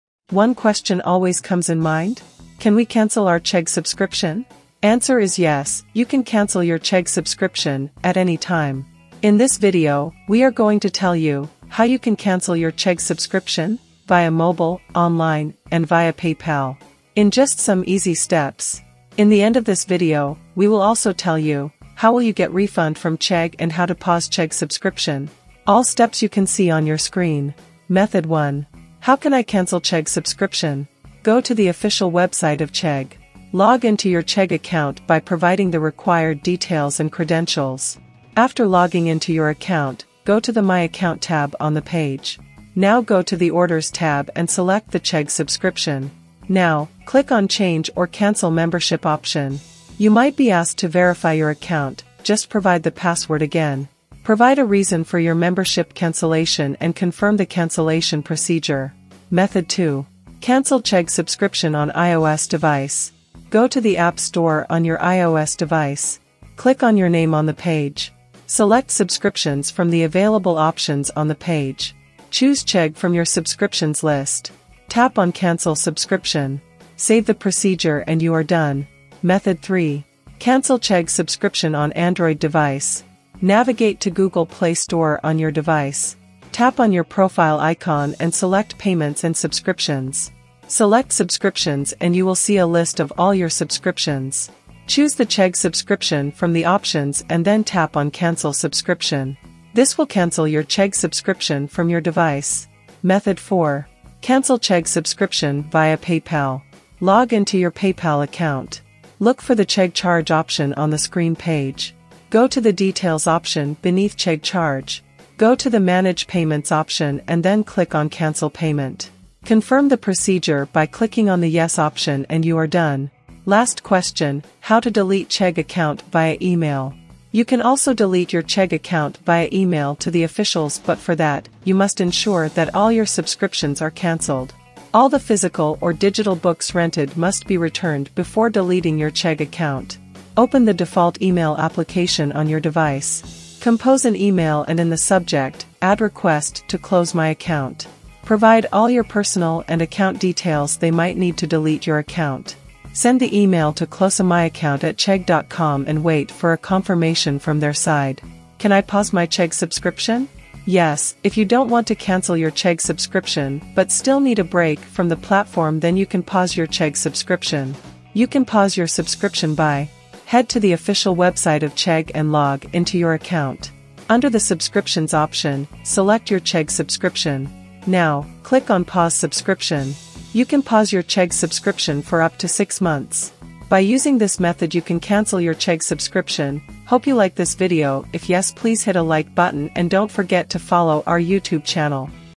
How To Cancel Chegg Subscription
cancel chegg subscription, how to cancel chegg,
cancel chegg, cancel chegg membership,
how to cancel chegg account,

method one. How Can I Cancel Chegg Subscription?

Go to the official website of Chegg.

Log In to your Chegg account by providing the required details and credentials.

After logging in to your account, go to the My Account tab on the page.

Now go to the Orders tab and select the Chegg Subscription.

Now, click on Change/Cancel Membership option. You might be asked to verify your account, just provide the password again.

 Provide a reason for your membership cancellation and confirm the cancellation procedure.

Method two. Cancel Chegg Subscription On IOS Device

Go to the App Store on your iOS device.

Click on your name on the page.

Select Subscriptions from the available options on the page.

Choose Chegg from your Subscriptions list.

Tap on Cancel Subscription. Save the procedure and you are done.

Method three. Cancel Chegg Subscription On Android Device

Navigate to Google Play Store on your device.

Tap on your Profile icon and select Payments and Subscriptions.

Select Subscriptions and you will see a list of all your subscriptions.

Choose the Chegg Subscription from the options and then tap on Cancel Subscription.

This will cancel your Chegg subscription from your device.

Look for the Chegg Charge option on the screen page.

Go to the Details option beneath Chegg Charge.

Go to the Manage Payments option and then click on Cancel Payment.

Confirm the procedure by clicking on the Yes option and you are done.